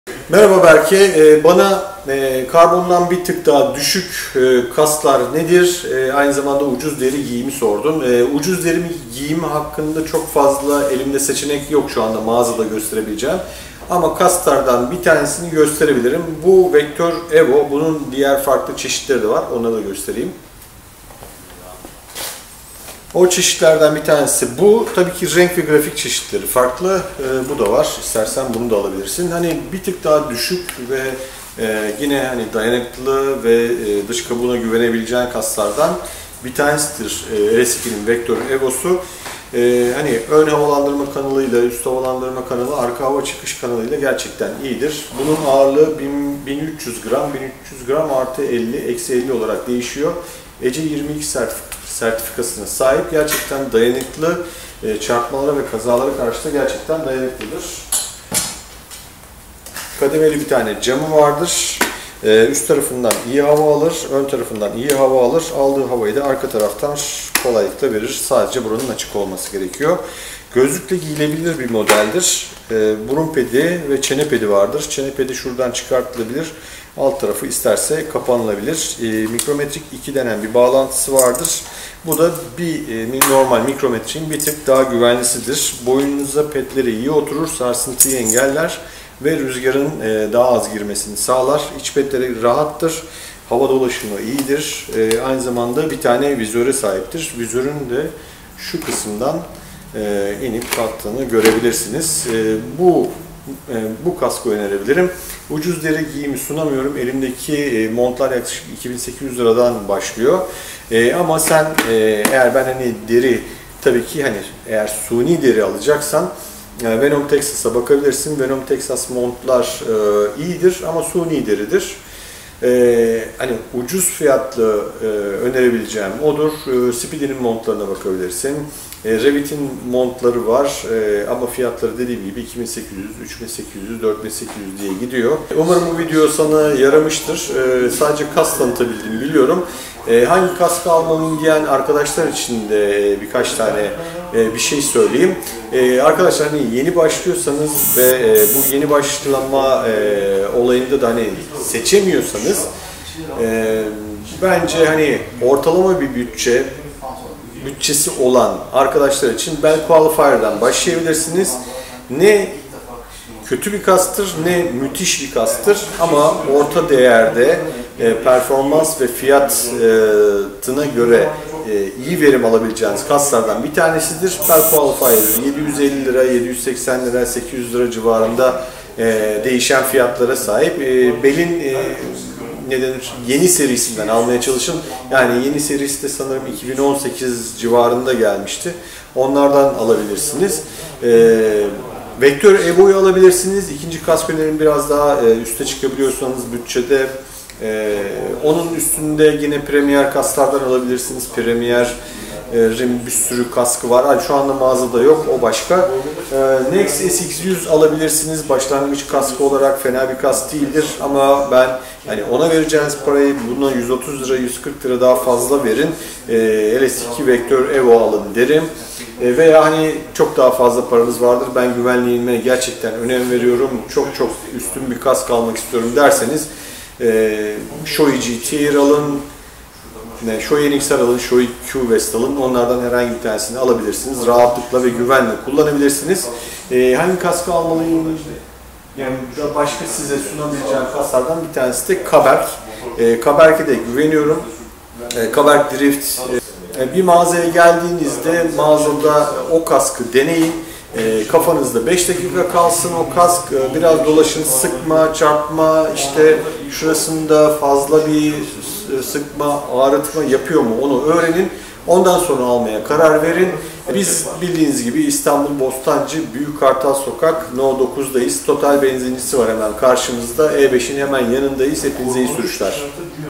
Başlangıç için Kask ve Deri Mont tavsiyesi
Başlangıç Kaskı ve Deri Mont tavsiyesi ürünler için aşağıdaki linklerinden detaylarına bakabilirsiniz. Bütçenize göre seçenekleri aşağıda bulabilirsiniz.

Bell Qualifier Torque Gold Kırmızı Kapalı Motosiklet Kaskı 765 tl

SHOEI QWEST BLOODFLOW TC-5 KASK 2750 tl

LS2 VECTOR VANTAGE MAT SIYAH-NEON SARI KASK 1300 tl

NEXX SX.100 SUPERSPEED MAT MAVI-YESIL KASK 939 tl

Macna Blast Deri Motosiklet Montu 1999 tl